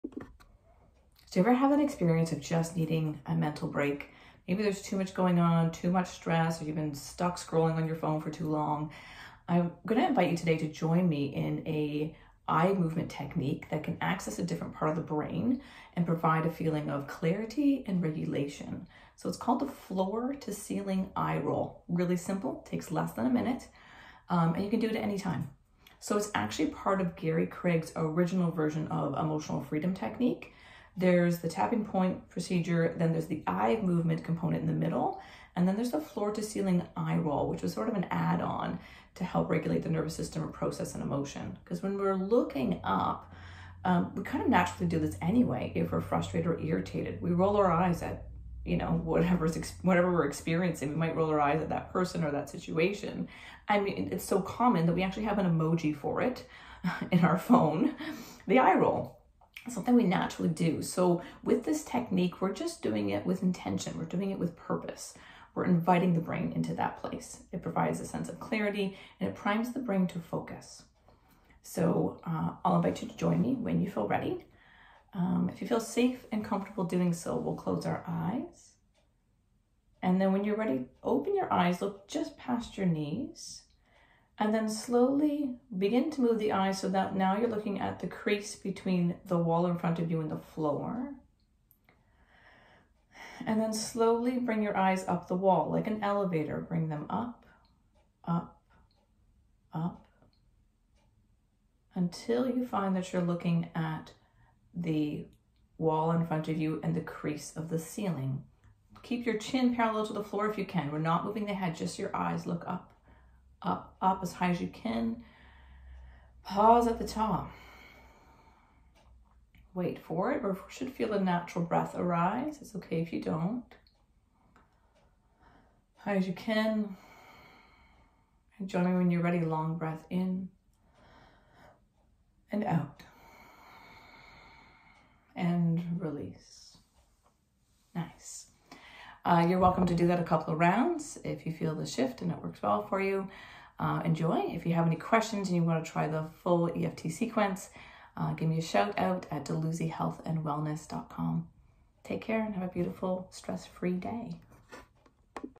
Coping Skill for Stress - Floor to Ceiling Eye Roll
Too much stress? Need a mental break? Try this “Floor to Ceiling Eye Roll” 🙄 🧠
It primes the brain to focus and helps the nervous system regulate and process emotions. It’s part of Gary Craig’s original version of Emotional Freedom Technique (EFT). We, as humans, naturally roll our eyes when we’re upset and this process is just intentionally engaging in that movement. If you have any questions just reach out